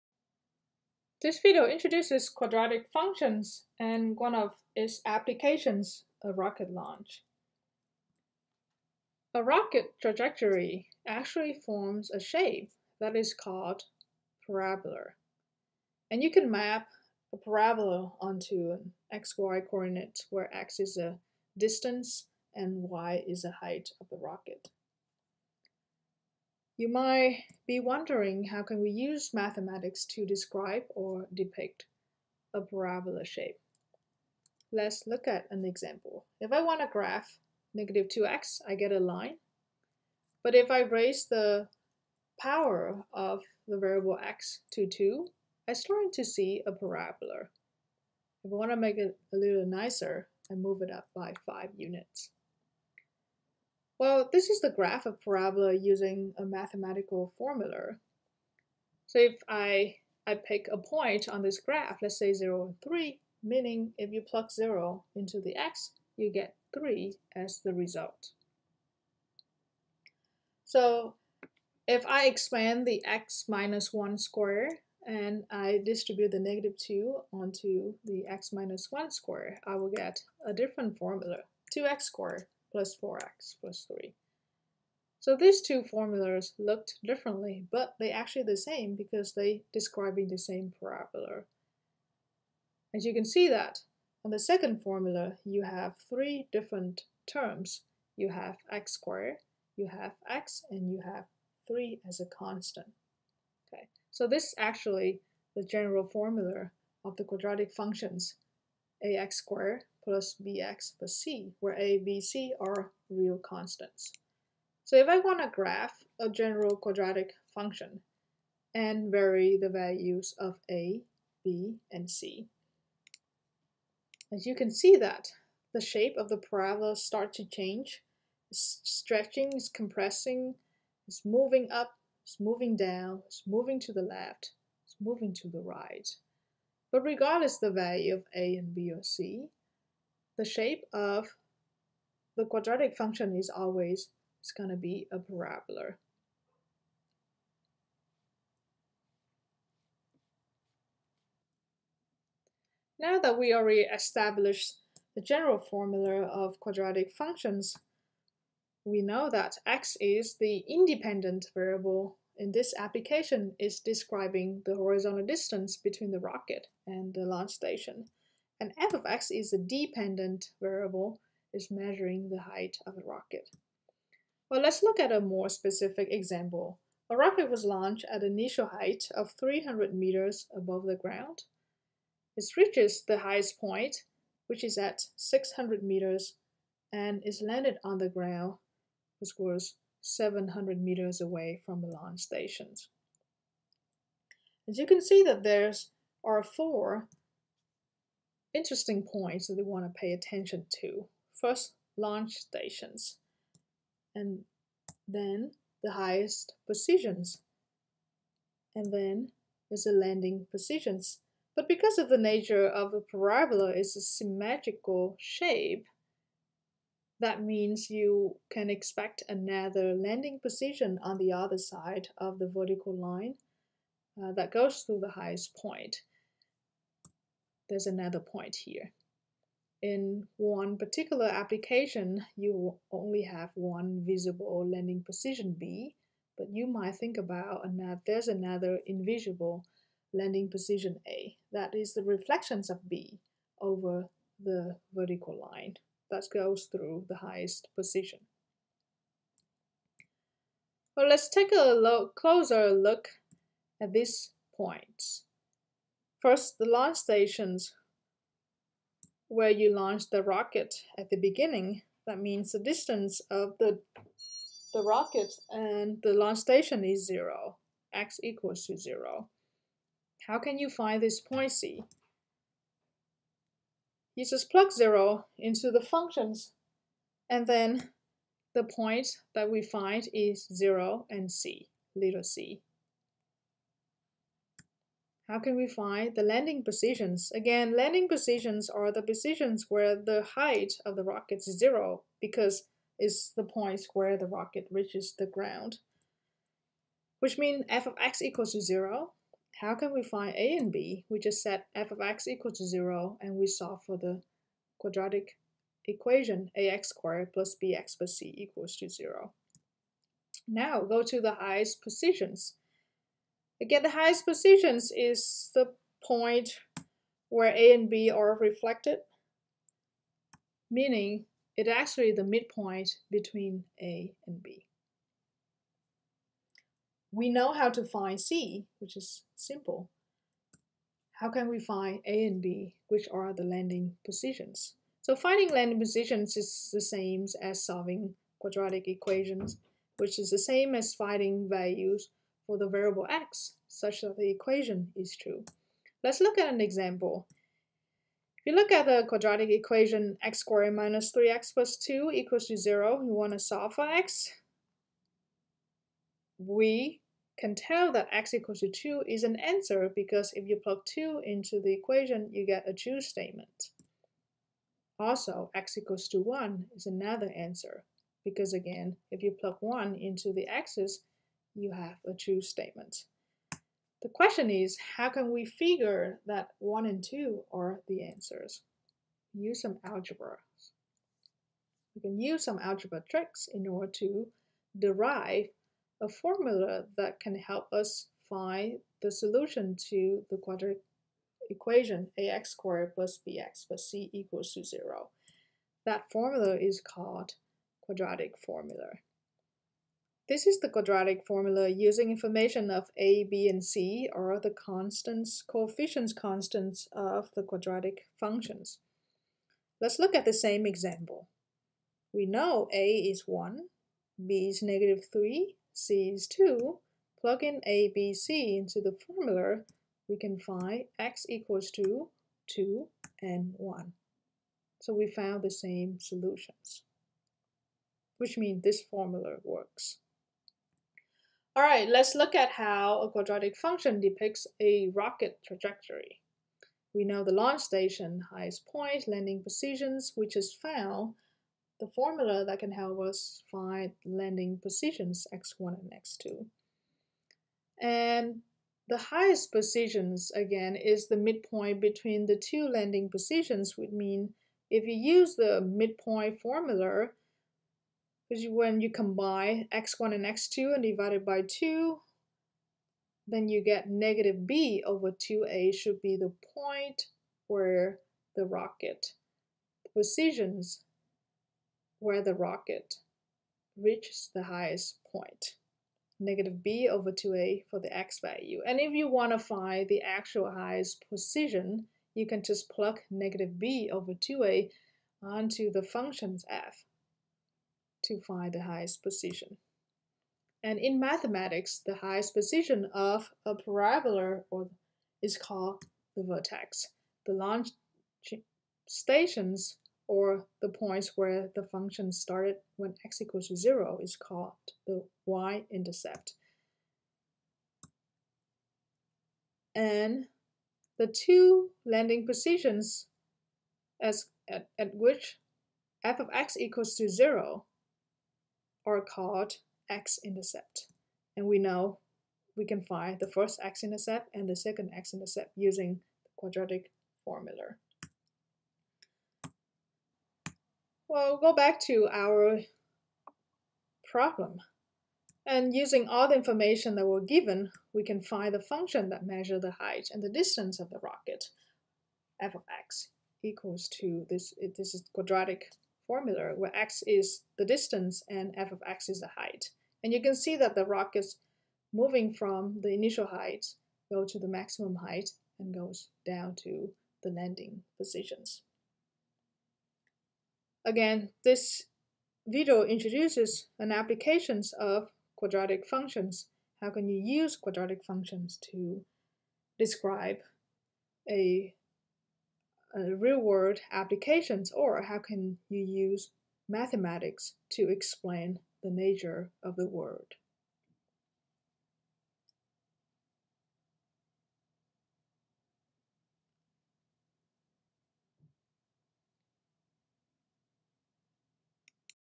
An Application of Quadratic Functions: Describing a Rocket Trajectory
This video introduce quadratic functions and one of its applications: describing a rocket trajectory.